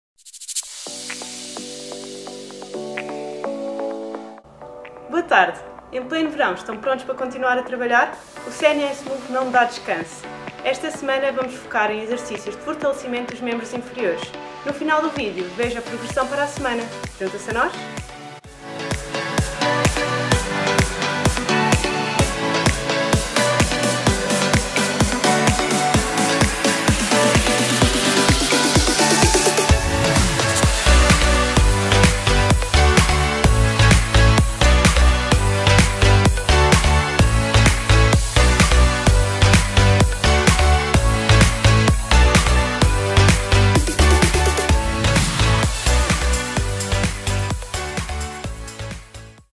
Desafio #32 | CNS move!
Bem-vindo ao desafio #32 do CNS move.
Convidamos a realizar os exercícios propostos e a partilhar connosco a sua experiência.
Está preparado? O seu treino começa agora.